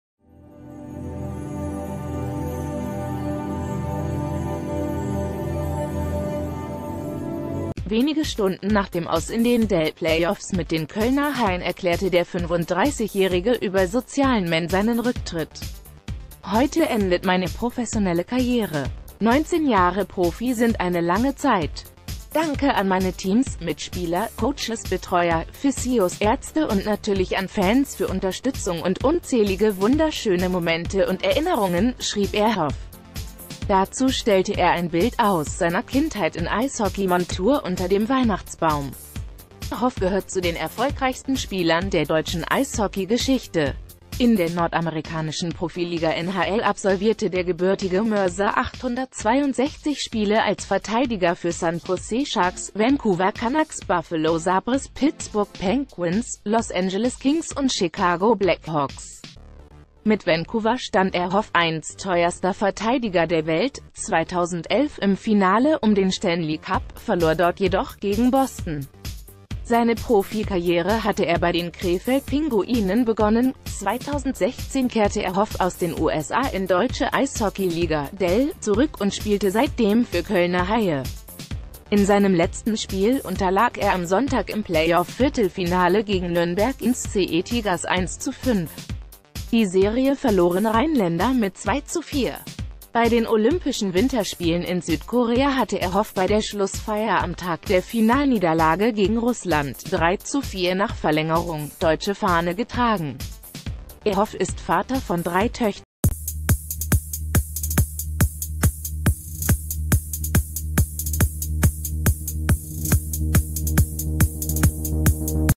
Aktuelle Nachrichten | Einen Monat nach Olympia-Silber: Ehrhoff beendet Eishockey-Karriere
Wenige Stunden nach dem Aus in den DEL-Play-offs mit den Kölner Haien erklärte der 35-Jährige über  Sozialen Men seinen Rücktritt. "Heute endet meine professionelle Karriere! 19 Jahre Profi sind eine lange Zeit. Danke an meine Teams, Mitspieler, Coaches, Betreuer, Physios, Ärzte und natürlich an  Fans für  Unterstützung und unzählige wunderschöne Momente und Erinnerungen", schrieb Ehrhoff. Dazu stellte er ein Bild aus seiner Kindheit in Eishockey-Montur unter dem Weihnachtsbaum. Ehrhoff gehört z...